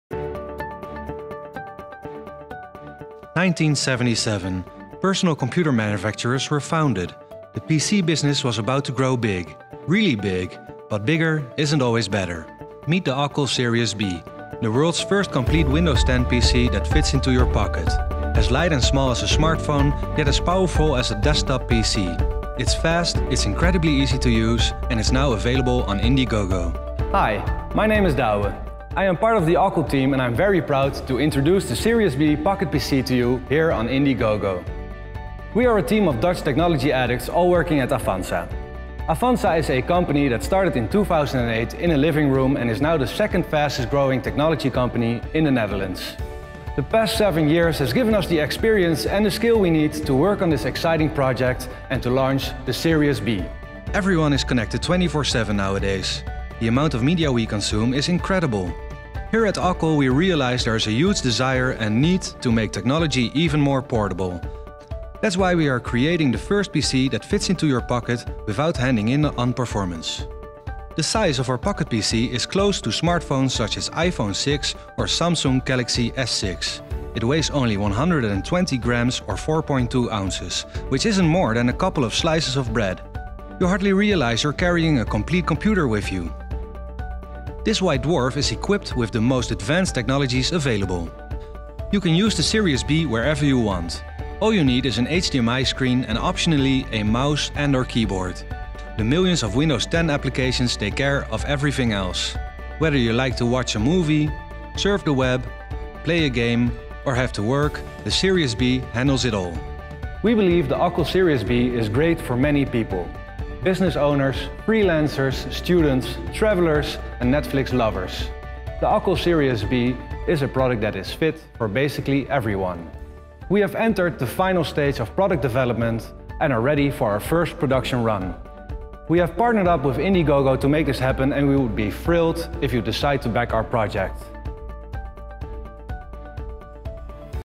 Ockel Sirius B Un PC de poche portable et léger comme un smartphone.
Un PC de poche, comme portable et léger comme un smartphone, mais aussi puissant qu'un PC de bureau.

Equipé du système d'exploitation le plus puissant du monde d'aujourd'hui, Windows 10, pratique à utiliser et magnifiquement conçu. Le processeur ultra-rapide quad-core Intel vous assure de suffisamment de puissance pour utiliser Sirius B pour toute tâche.

Le Ockel Sirius B est  maintenant disponible sur Indiegogo . Rejoignez - nous et faire partie de la prochaine étape dans la technologie portable.

Ockel Sirius B Un PC de poche aussi puissant qu'un ordinateur de bureau et aussi portable et léger comme un smartphone.